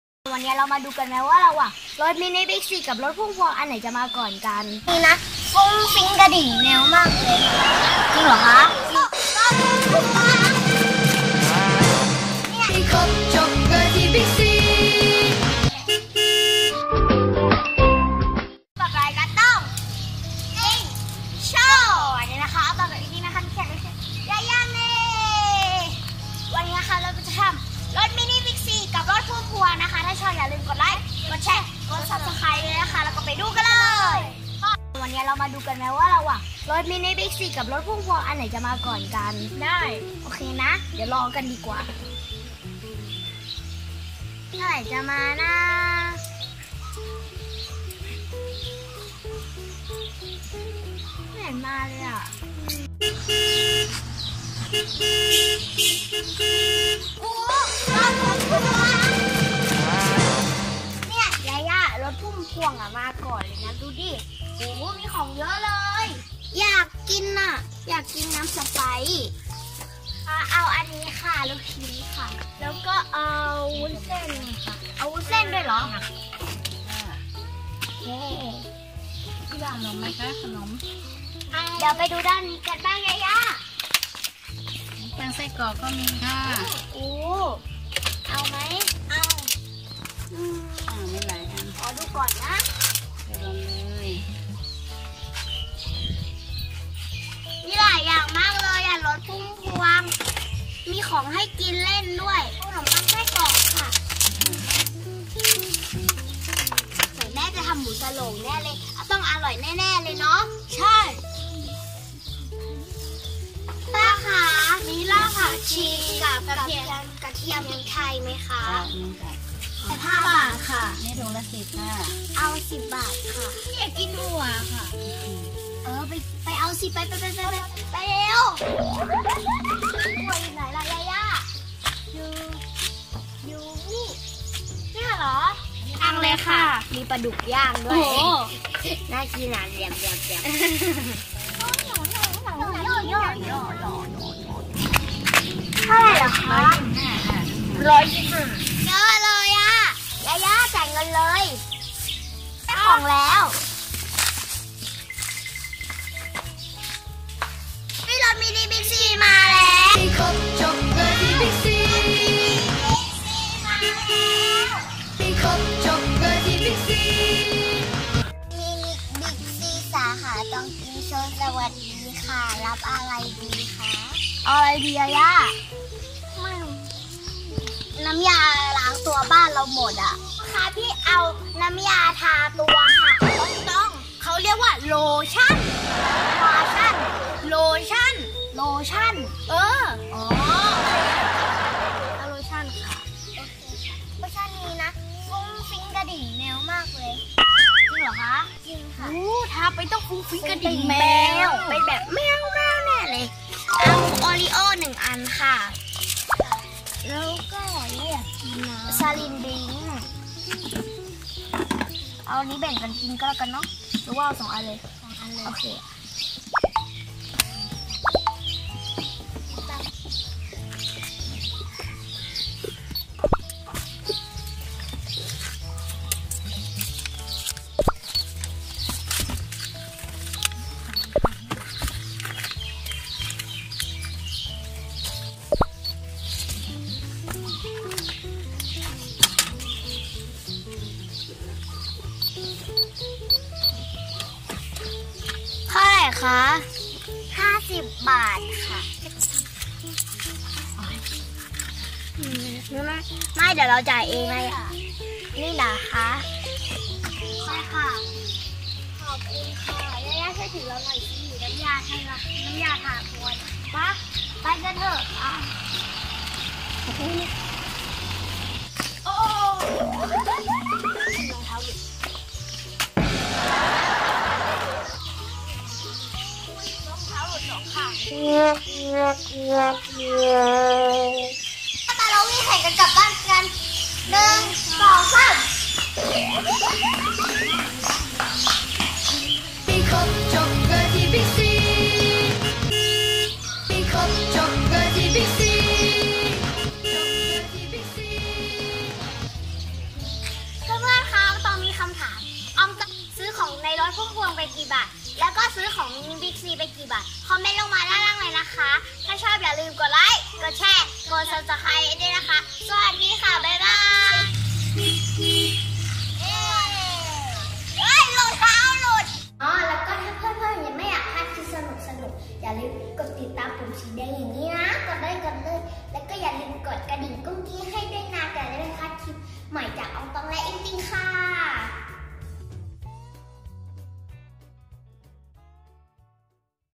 รถมินิบิ๊กซี vs รถพุ่มพวง | กล่องกระดาษ E.p.24 ♥️ ตอง ติง โชว์ ♥️
ตองติงแชนแนล ตองติงเซเว่น ตองติงละคร ตองติงโชว์ นิทานตองติงโชว์ ตองติง ตองติงเซเว่น อิงติงโชว์ อองตองโชว์ ซีรีส์ชามยักษ์ตองติงโชว์ รถขายของตามหมู่บ้านในตรอกซอกซอก จะมีรถเร่ หรือที่เราเรียกกันทั่วไปว่ารถพุ่มพวง และ รถมินิบิ๊กซี เราจึงนำรถ 2ประเภทมาเปรียบเทียบกันให้ดู คลิปนี้สร้างขึ้นเพื่อความบันเทิงเท่านั้น

5 เหตุการณ์เงิบๆ!! ที่ต้องเคยเจอในเซเว่น